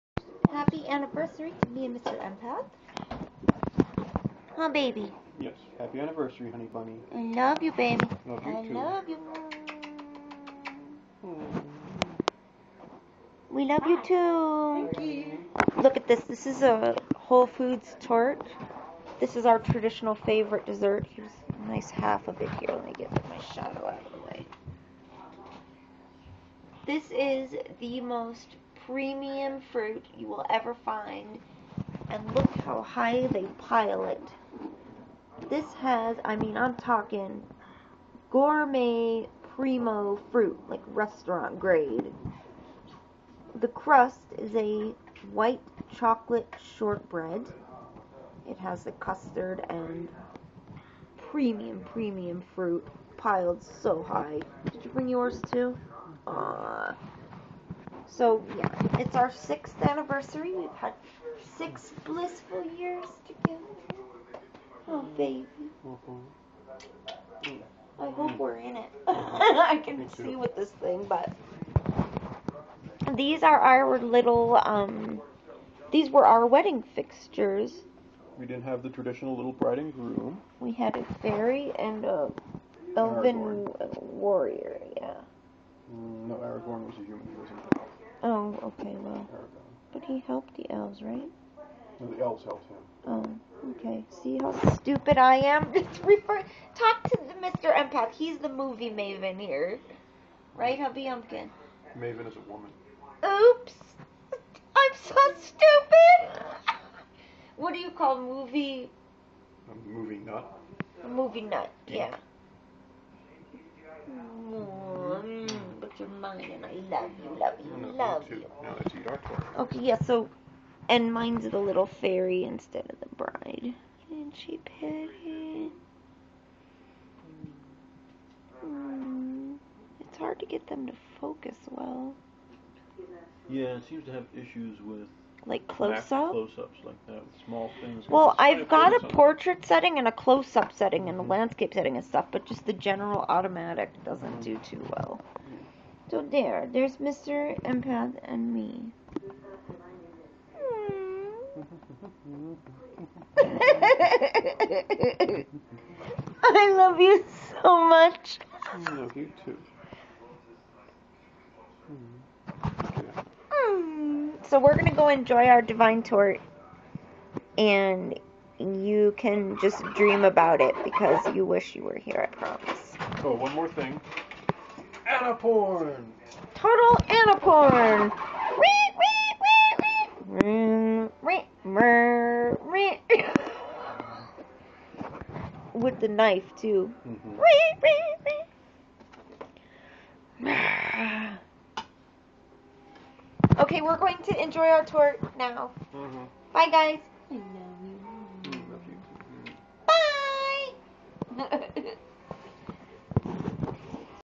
AnaGirl & Mr. Empath's 6th Anniversary!
Hey, everyone!  Mr. Empath and I wanted to share a few moments with you from our Anniversary celebration. We are such giddy, happy spirits; elated to have found each other in a tumultuous sea of vast disappointments. Our message? Anyone can find true love. You couldn't have convinced either of us before we met. I myself was a lonely, depressed virgin and sure I was going to die that way before I met my other half.  I was 29 when we met.   We had been so hurt in toxic relationships and years alone after that. Now we are so grateful and never take a single day for granted! :)
I share our little figurines from our wedding (I'm a fairy; he's a warrior from LOTR).
Lastly, Mr. Empath deems the torte gratuitous Ana porn!  :D  No video with both food and Mr. Empath in it could pass without us making the Ana Porn joke... so he adds that at the end! ^_^  I LOVE HIM SO MUCH! :)